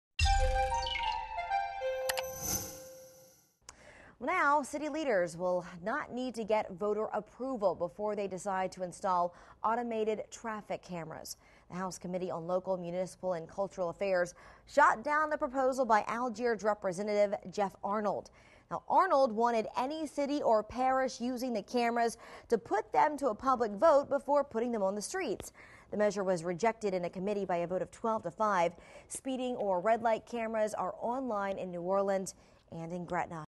House Committee Rejects Traffic Camera Bill
A bill that would have allowed the public to vote before a city installs automated traffic cameras is now dead.